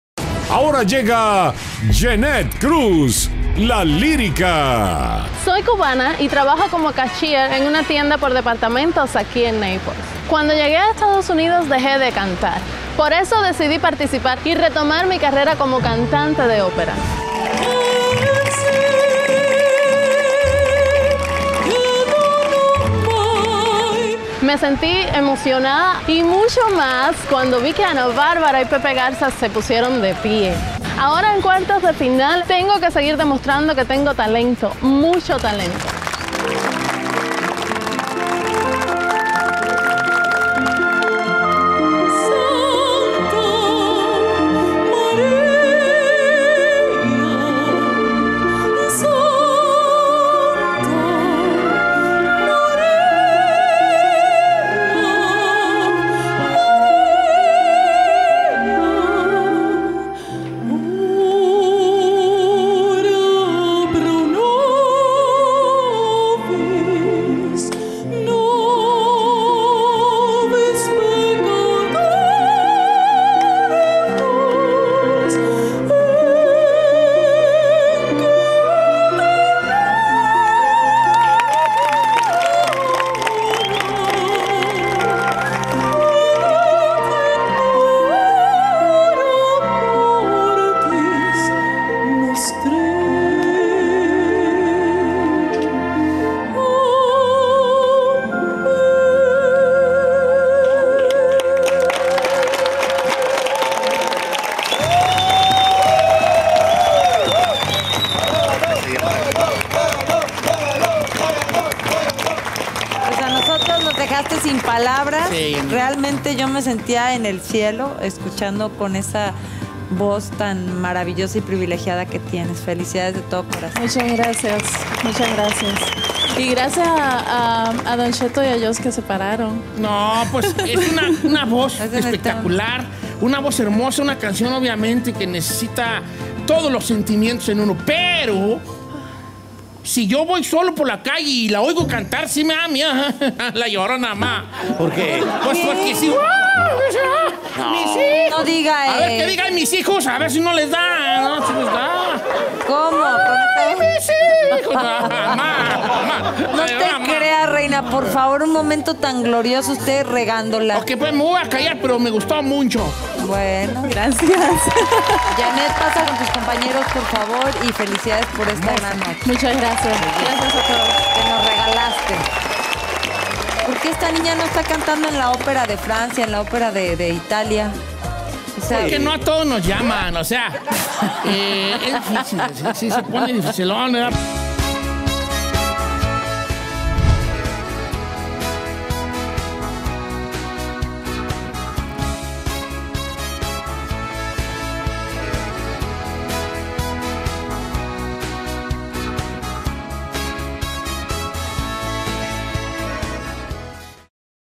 DON CHETO CONMOVIDO CON LA VOZ DE YENET CRUZ
Al igual que Don Cheto, todos los jueces terminan de pie ante la preciosa voz de Yenet Cruz!